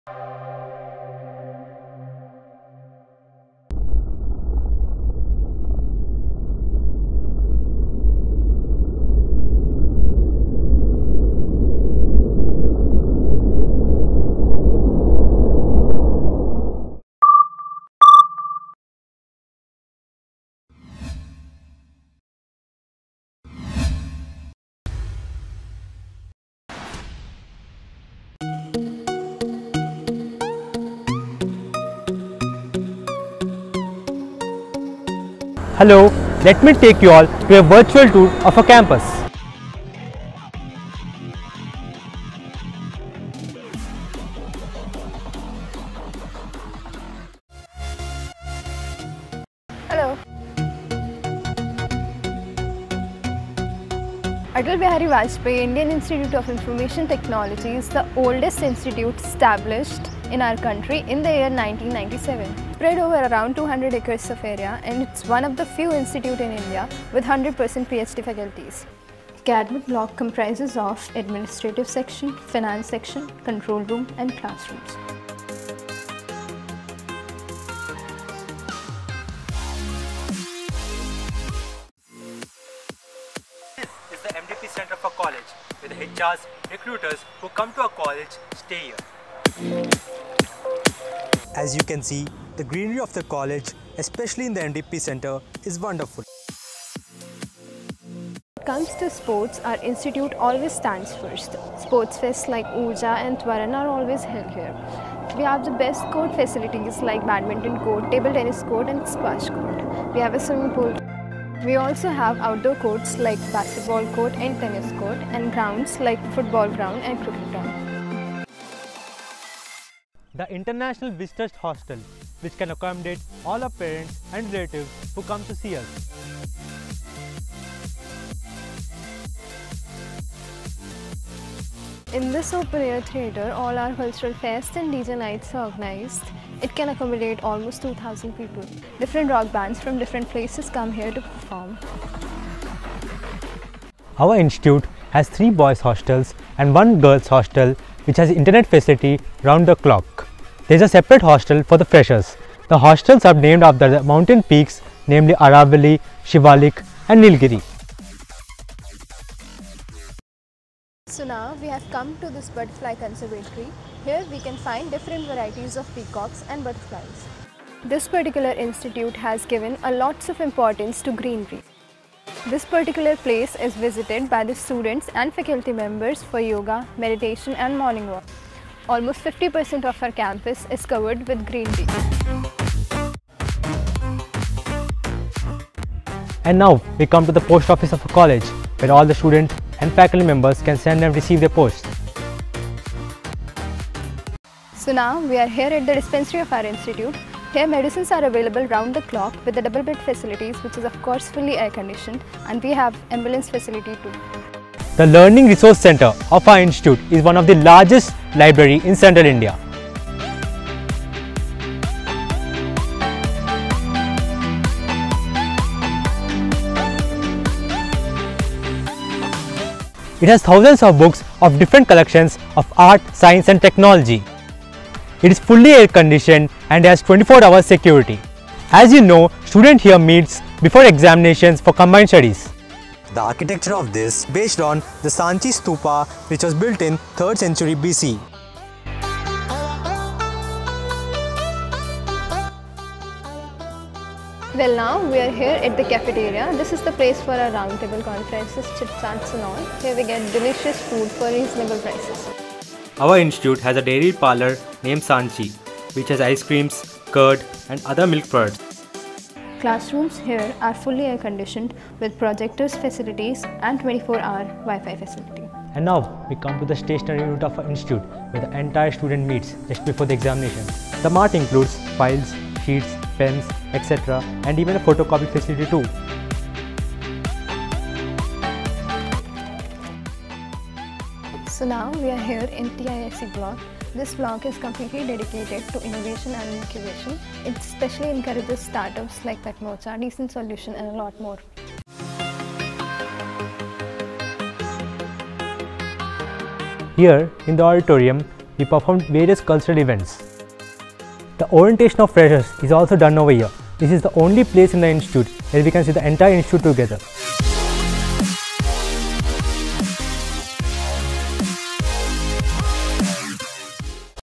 ABV - IIITM GWALIOR CAMPUS TOUR
This is the campus tour of our institute which is the first IIIT set up in our country.  The video covers the major infrastructures and places of the institute.

Cast : Girish Suragani || Priya Yadav
Editing - Cinematography - Screenplay  - Direction : Pavan Praneeth